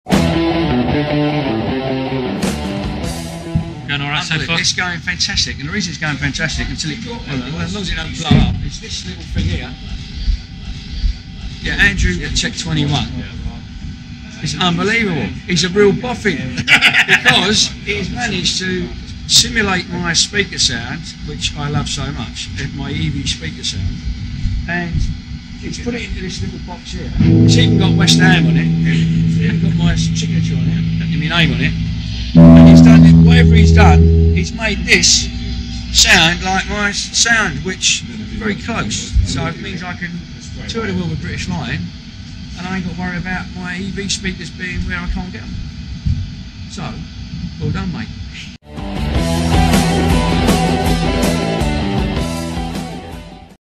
Steve Harris Behind the Scenes: British Lion Rehearsal
Steve's reaction when he first tried the SH1 Signature SansAmp prototype at a British Lion rehearsal. (Subtitles are provided due to the poor audio quality.)
For more insight on how the Steve Harris SH1 Signature SansAmp came to be, check out our full interview with his tech and Iron Maiden keyboardist, Michael Kenney: